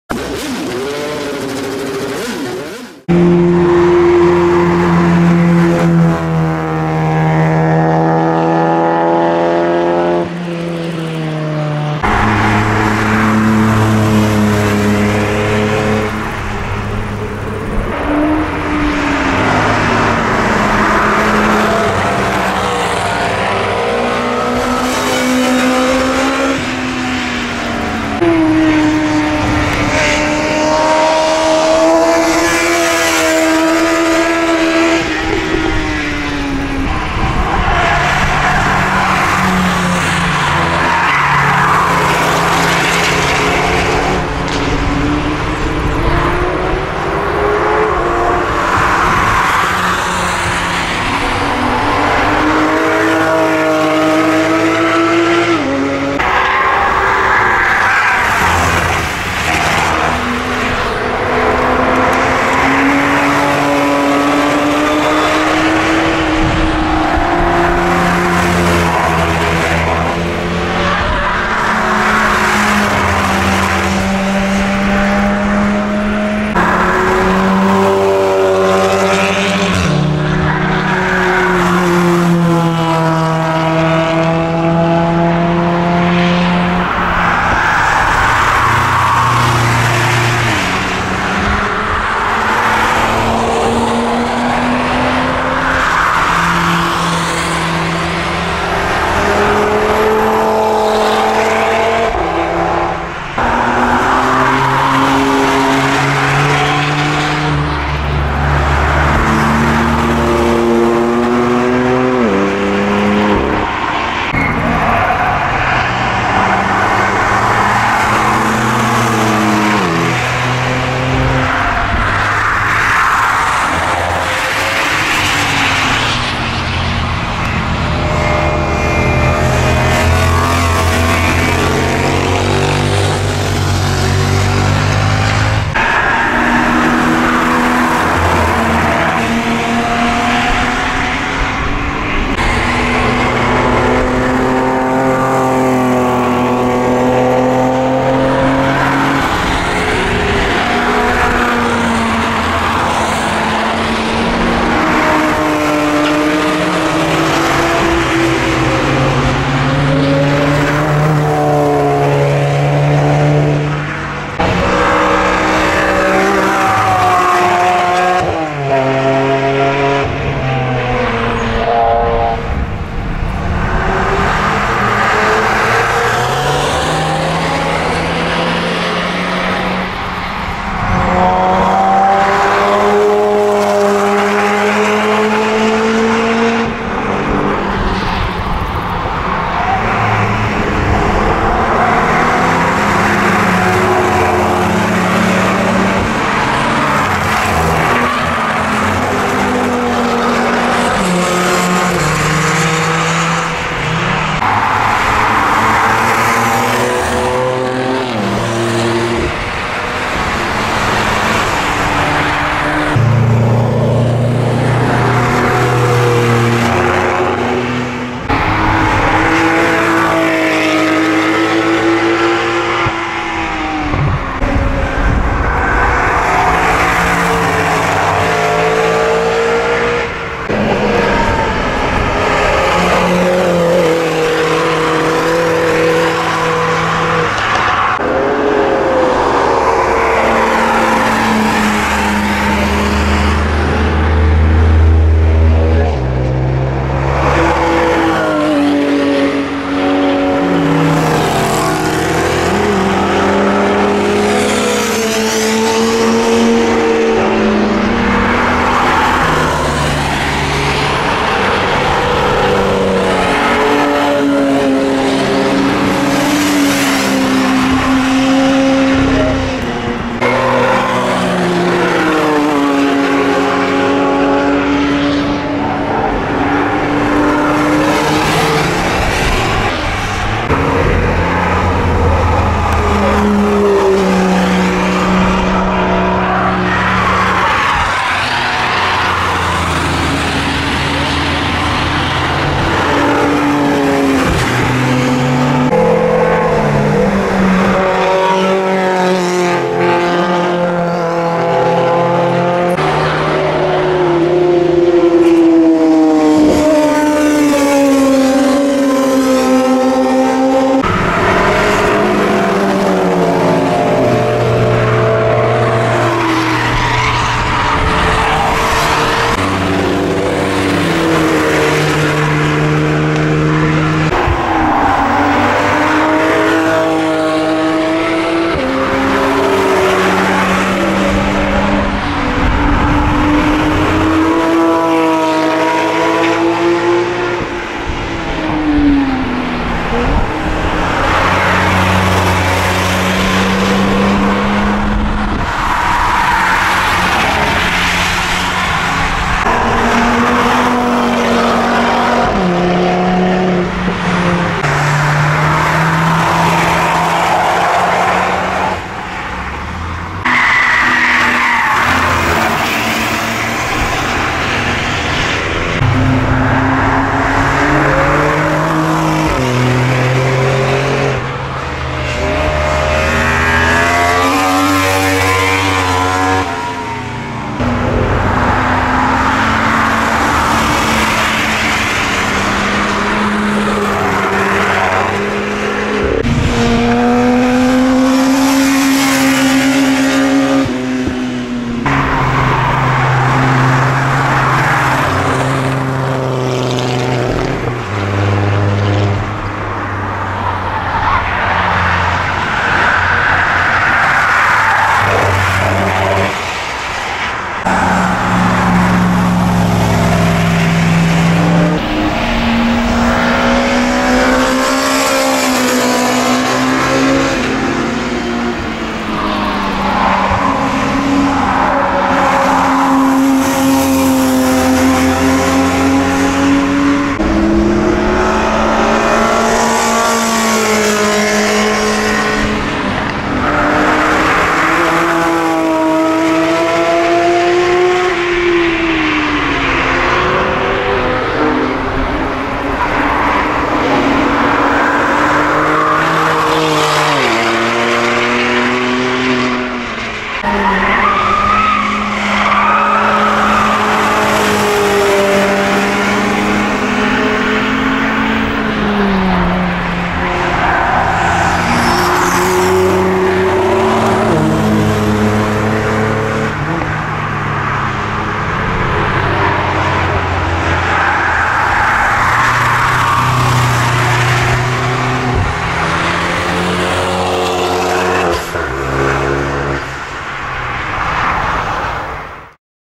Touristenfahrten Nürburgring Nordschleife 09.08.2022
Ein paar Ausschnitte von den Touristenfahrten an der Nürburgring Nordschleife vom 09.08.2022